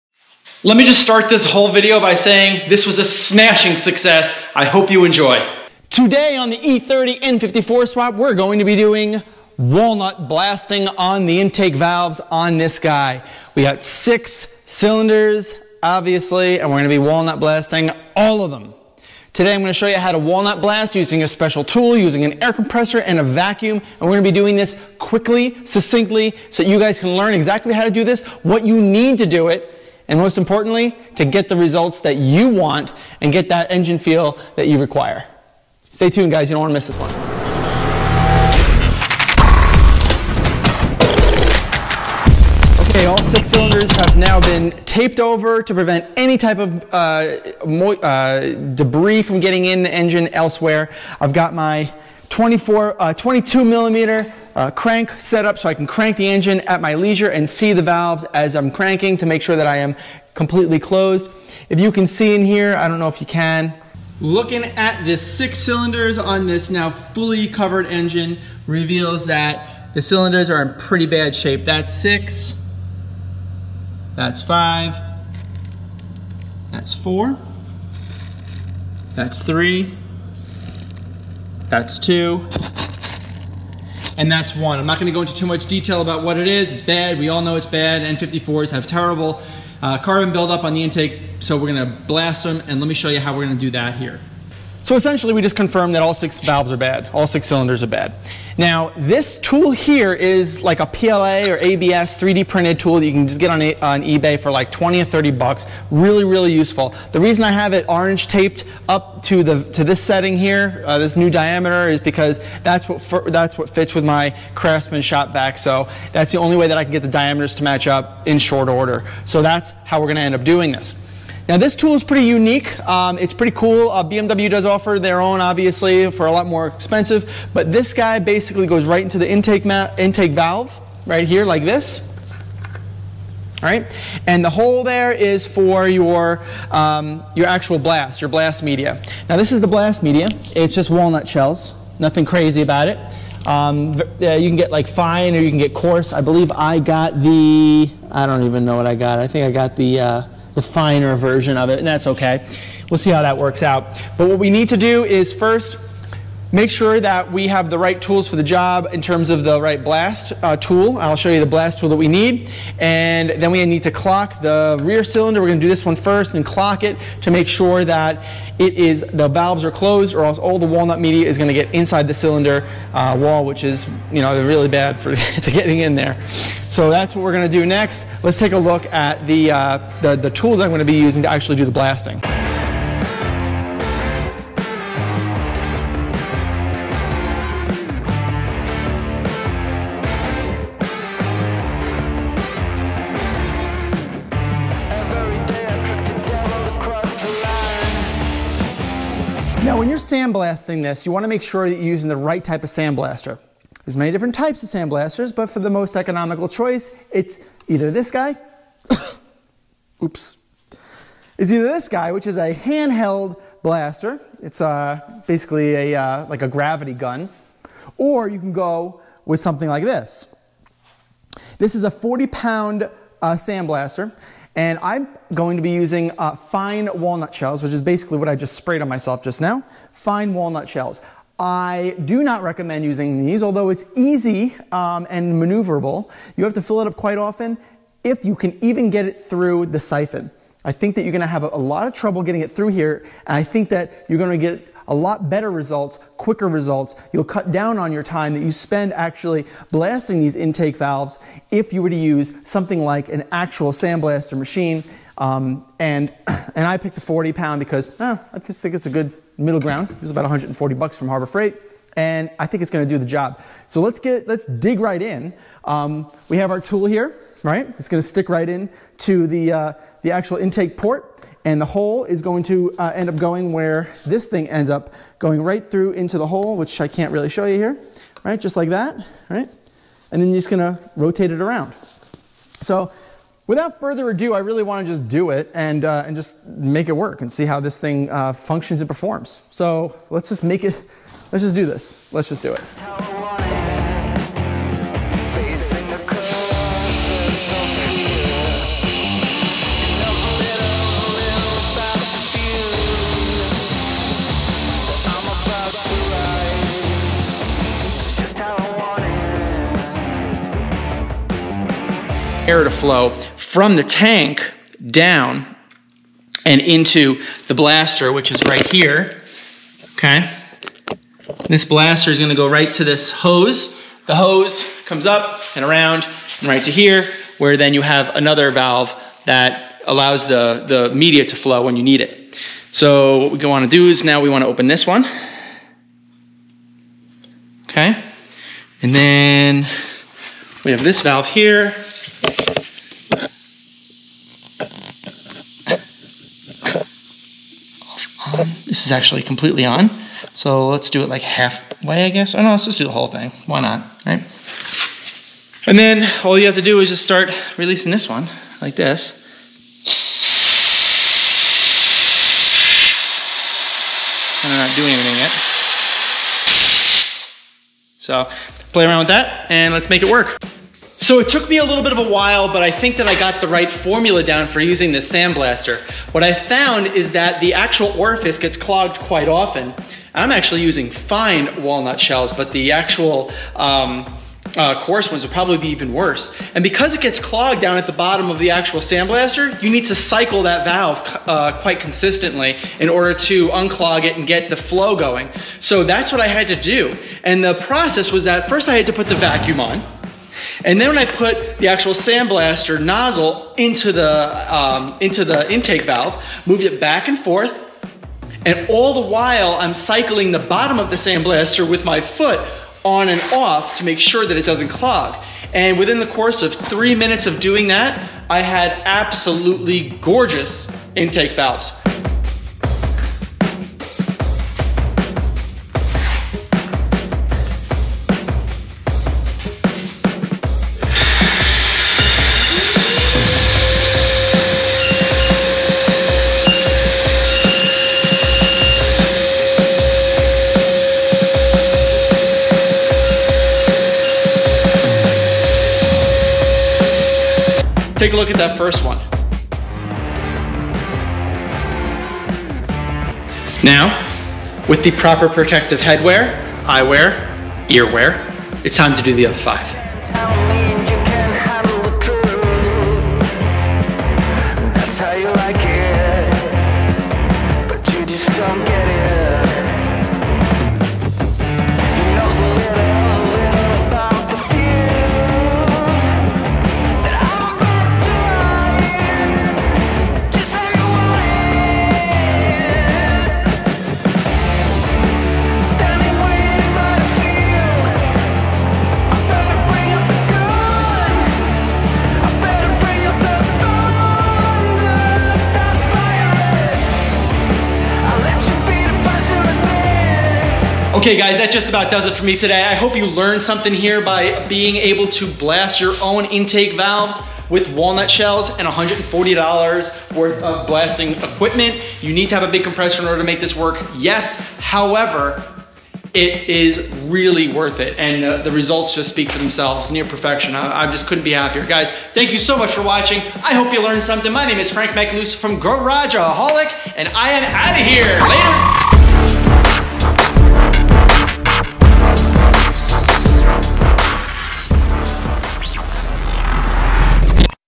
Part 19: How To REALLY Walnut Blast Intake Valves - Concise How-To for Any N54 Swap!
In Today's Video, I concisely and easily explain how to effectively walnut blast your intake valves. Using some special tools listed below, you can blast your own intake valves! This took some trial and error to get it right, but after I got the hang of the first cylinder, all of the other 5 cylinders literally took me just a solid 20 minutes to complete.

You need the following:
-Shop Vacuum capable of sucking some serious walnut shells
-Fine walnut shells. 25lbs from harbor freight is more than enough
-Abrasive Blaster - They come in 20, 40, or 110lbs from Harbor Freight. I used the 40lb for this job.
-$24 walnut blaster attachment tool for N54/N55
-You must have at least a 40 gallon compressor to keep up with the demand, or slowly do one cylinder at a time to allow your compressor to keep up.

E30 N54, E36 N54 Swap Guides available for purchase!